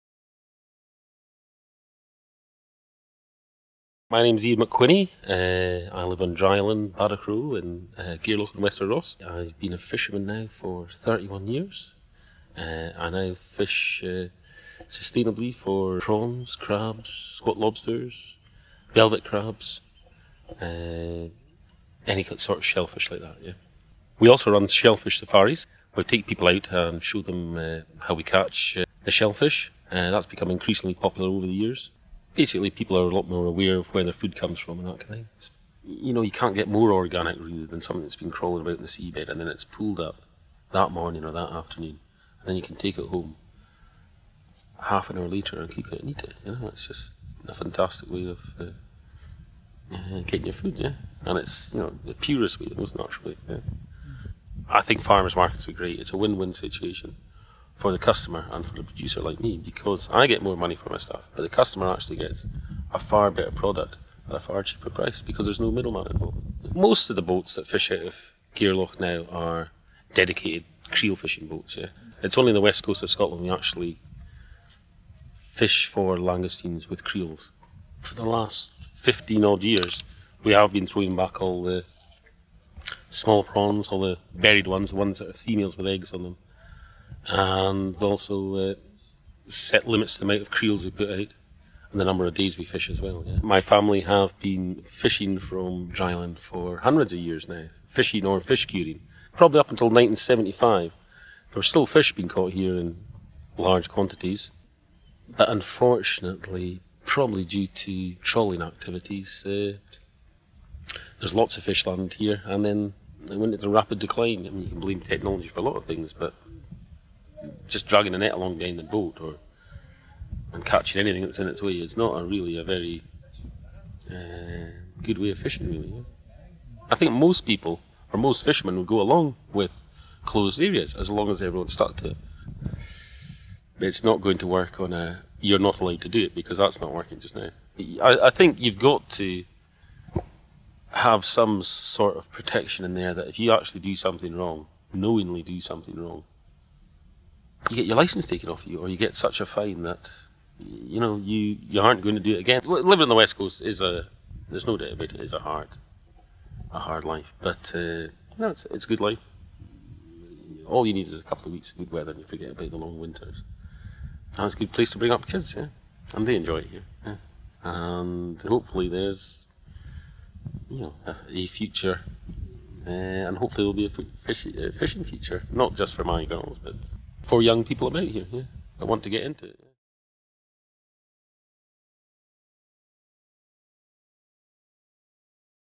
Sealife - the creel fisherman
A diverse range of livelihoods in Scotland are dependent on a healthy marine environment. Join our campaign for Marine Protected Areas

About this photo story - Ian McWhinney lives on Dry Island on Gairloch in Wester Ross. He has been a fisherman for 31 years, continuing an activity that has been in the family for centuries. Respect for the marine environment is important to him to ensure a future for fishing in this area.

About the collection - the Sealife photo story collection was produced by journalist and photographer Jamie Grant - visit jamiemurraygrant.co.uk for details.